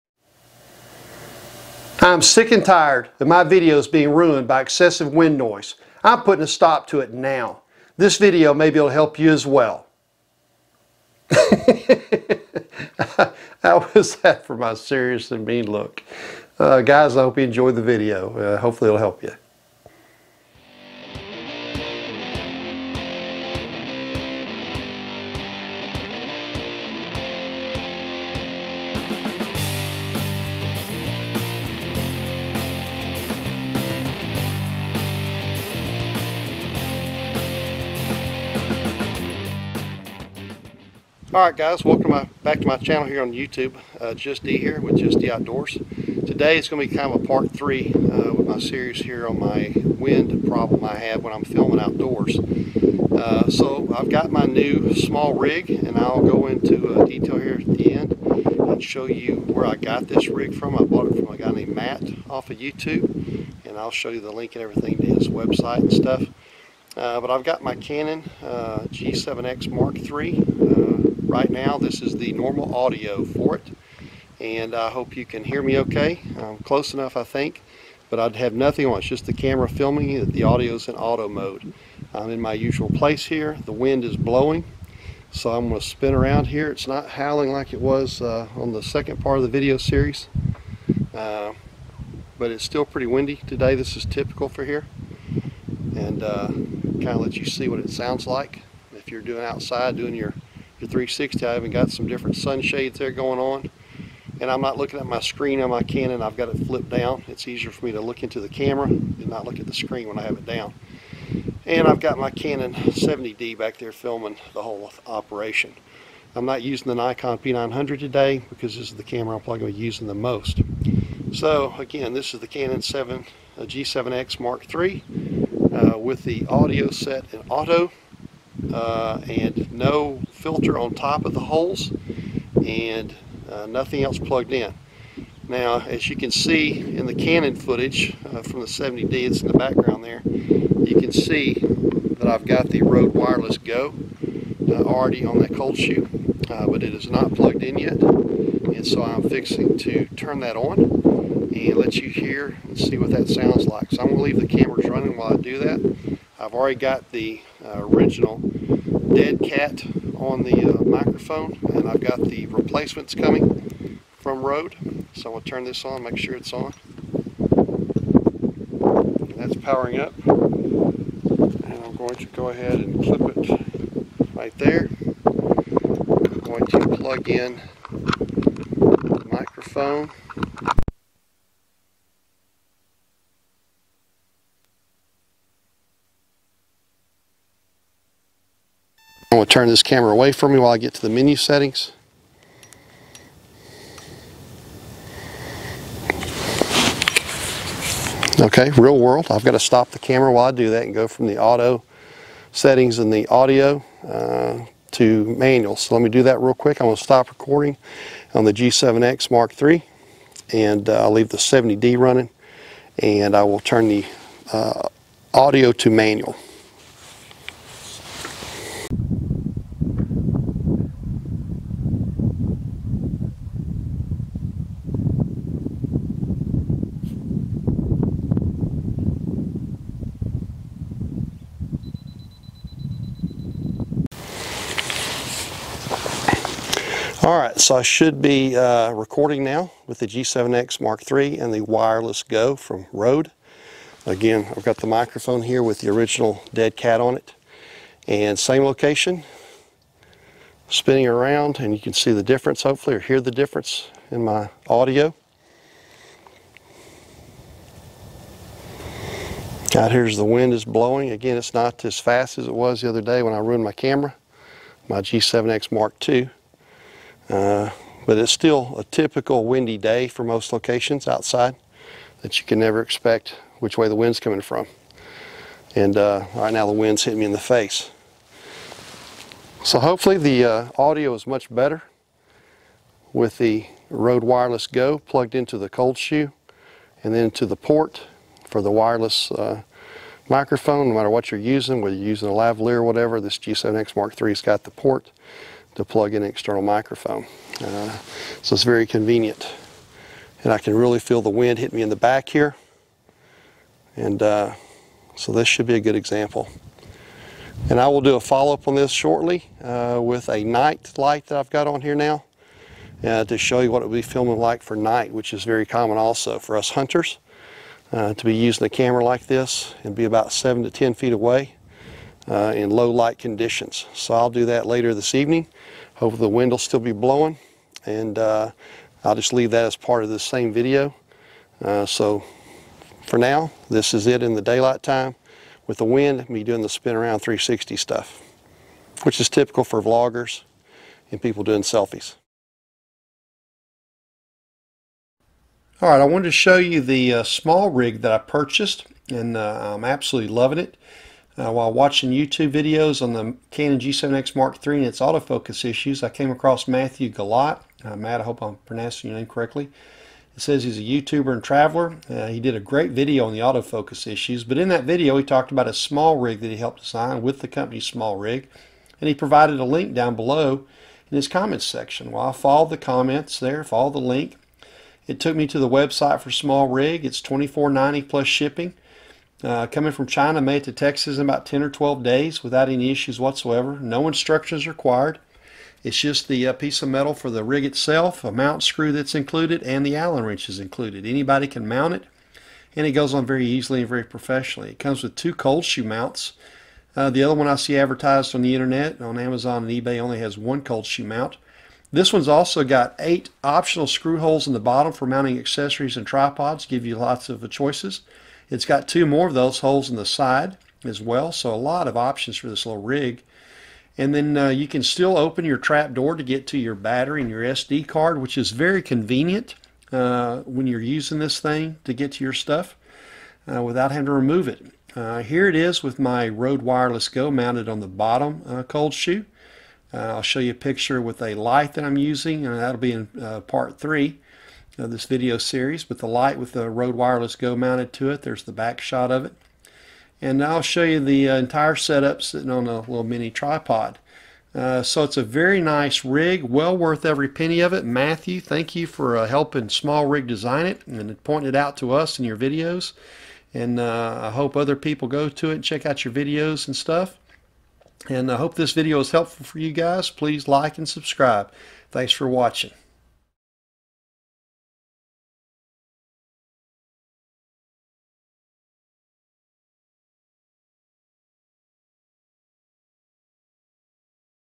Canon G7X Mk III, the RODE Wireless Go, and a Small Rig
Part 3 in the series on how to film in the outdoors.

In this video, we take a look at how you can defeat the wind in your videos.

The Canon G7X Mk III using a RODE Wireless Go mounted on a Small Rig designed by 'the JaYoe Nation'

His video reviewing the Canon G7X Mk III camera

The link to the Small Rig shown in my video, designed by Matthew Galat